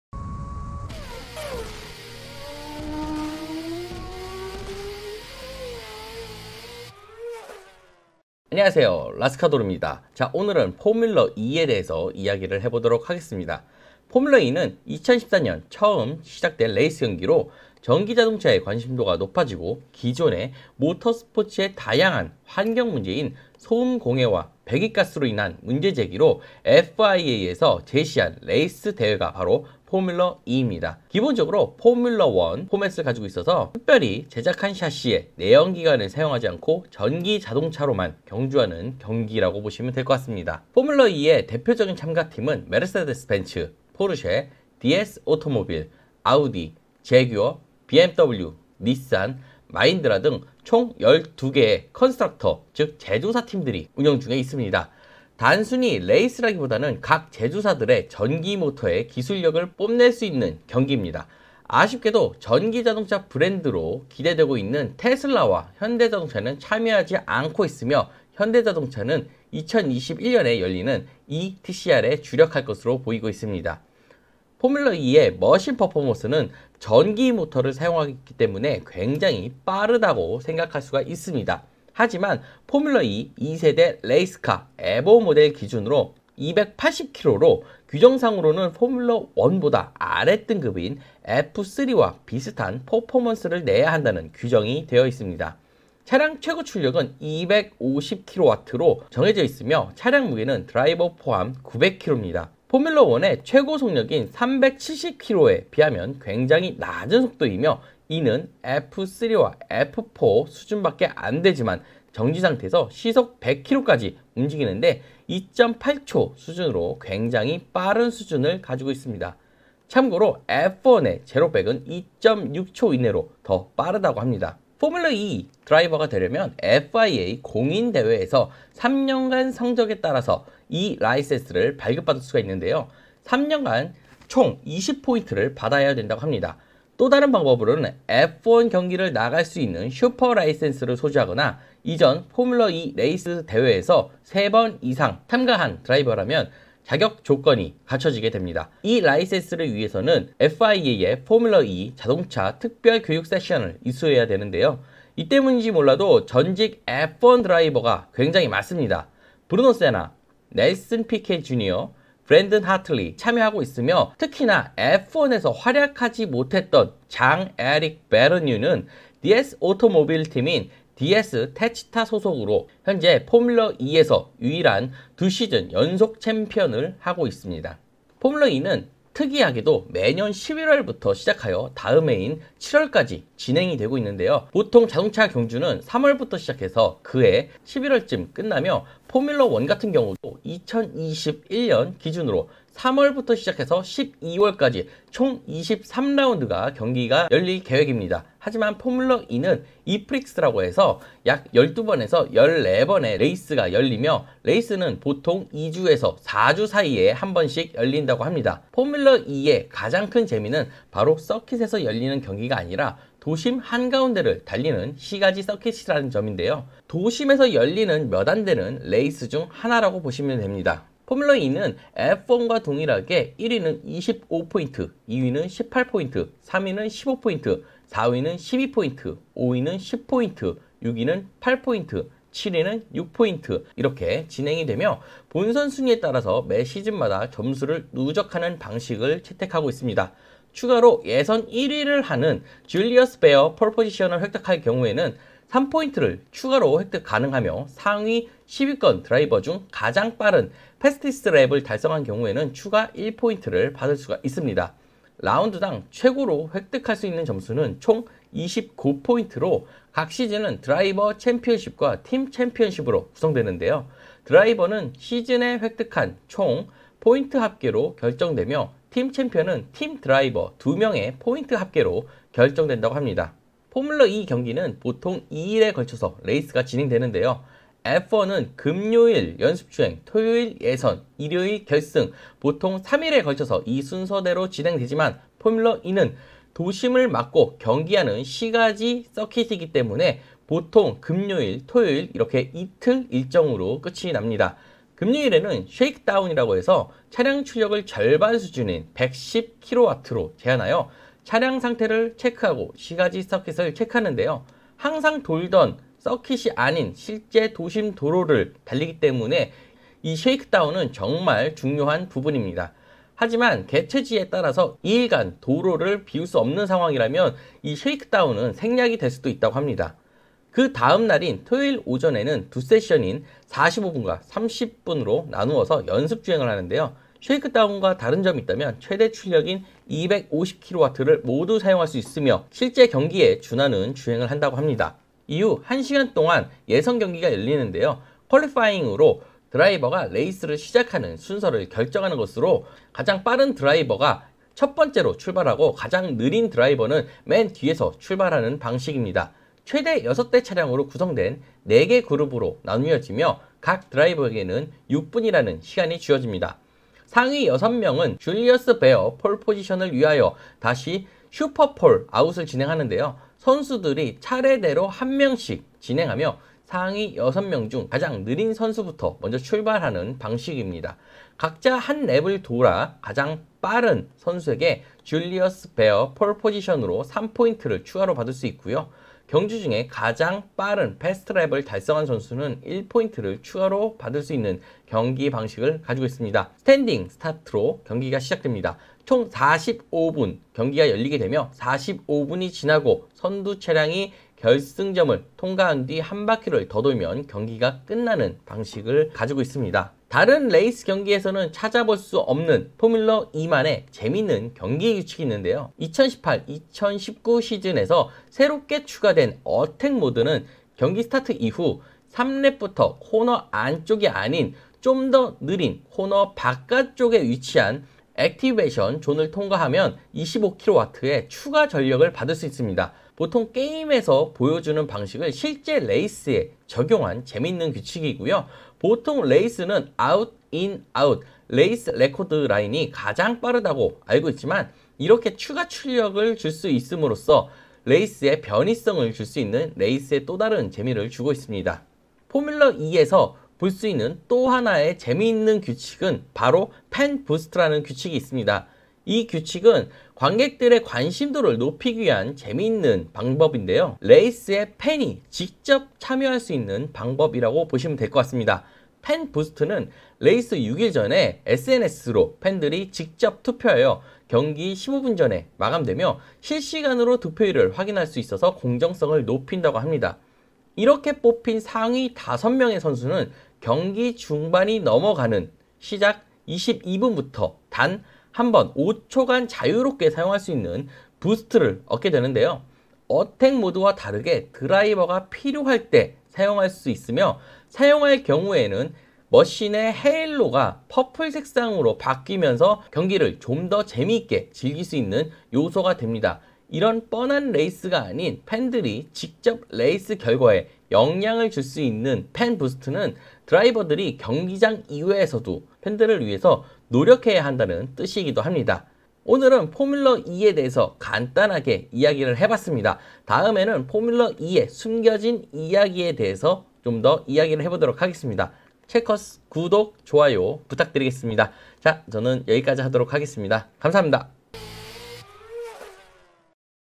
8분만에 알아보는 서울에서 개최될 뻔한 포뮬러 E 레이스 대회에 대해서 알아보자
2020년 서울에서 개최 예정이였던 포뮬러 E 경기단 8분 동안 전기 레이스카 경기에 대해서 보다 쉽게 이야기를 해봤습니다.

포뮬러E 최고속도는?? 제원은?? 어택모드?? 팬부스트 이 모든 내용을 함축해서 담았습니다.
제목은 앤드류의 5분 대박과사전 님을 오마주 해봤습니다.